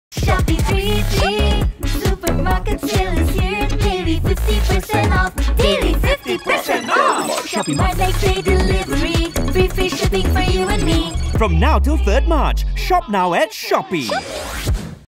Shopee 3.3 Supermarket Sales 15s Cutdown
Client: Shopee Malaysia
Job Title: Shopee 3.3 Supermarket Sales
Duration: 15s
Medium: Digital
Language: English
Audio House - Super Red Music Sdn. Bhd